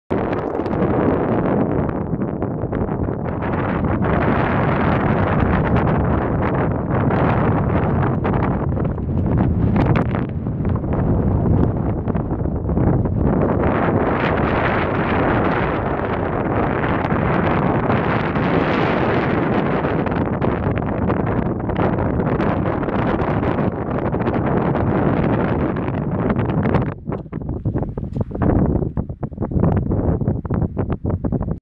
La desolazione de La Graciosa (Canarie)
La desolazione de La Graciosa (Canarie), il fascino di trovarsi in mezzo al niente a La Graciosa